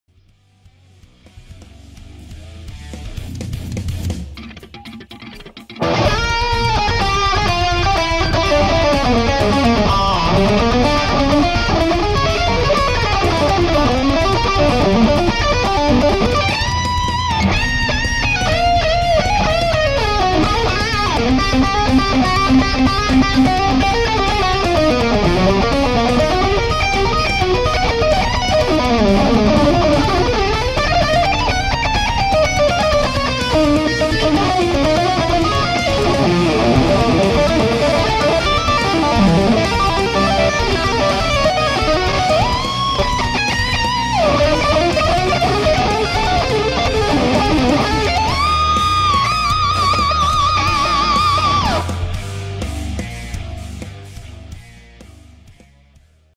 Abasi Neural Contest - 1ST Place Solo Cover - Unda Alunda
Hi! Last week I've been practicing that solo a lot :) Very interesting choices of notes, truly love this solo. Unda Alunda is a real master ;)